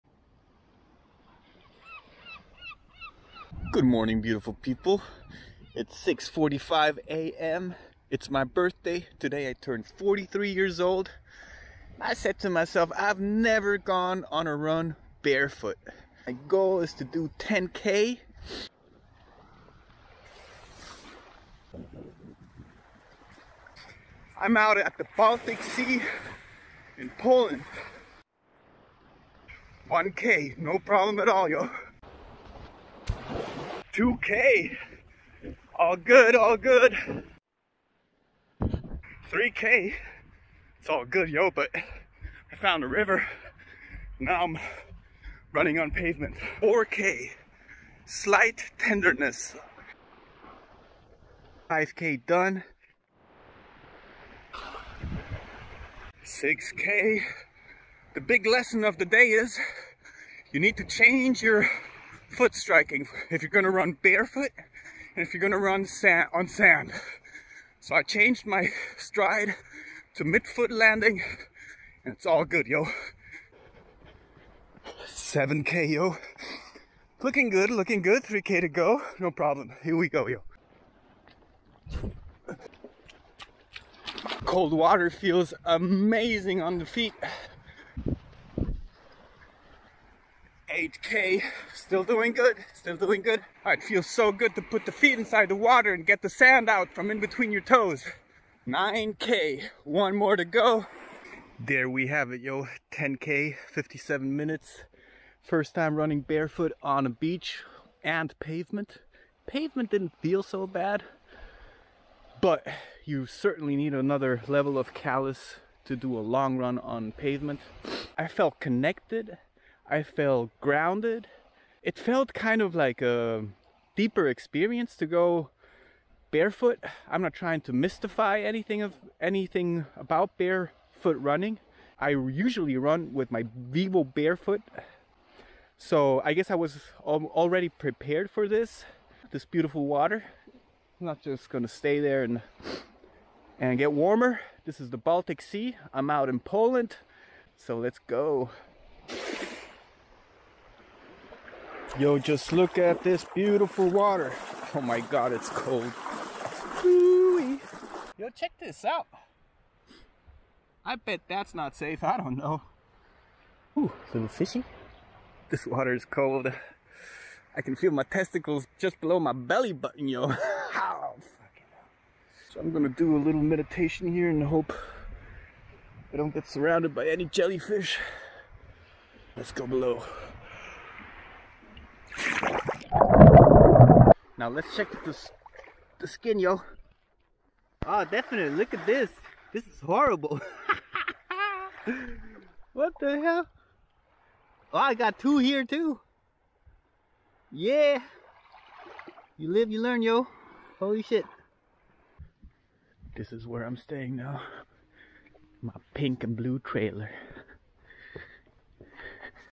Birthday Barefoot Adventure: My First 10k barefoot Run at 43! 🏃‍♂️🎉
Hey there, YouTube warriors! Today (actually yesterday, August 13th, but I have no internet in this village so it took a while to upload 😅) is a special day - it's my 43rd birthday, and what better way to celebrate than with a brand-new adventure? Currently, I'm in the charming village of Rogowo, Poland, right by the Baltic Sea. The soft white sand here looked inviting for a unique challenge: a barefoot 10k run. Yes, you heard that right - barefoot! I've never attempted a long run without my trusty vivobarefoot minimalist sneakers, so this was quite the experience. The beginning was a breeze, but things got interesting when I encountered a river and had to switch my stride. The sand's texture posed its own challenges, and I even earned a few battle scars in the form of blisters. But the lessons I learned were worth every step. With a finish time of 57 minutes, I may have been slower than usual, but considering it was my first barefoot run, I'm thrilled with the accomplishment. Join me as I reflect on this unique birthday run and the insights it brought me!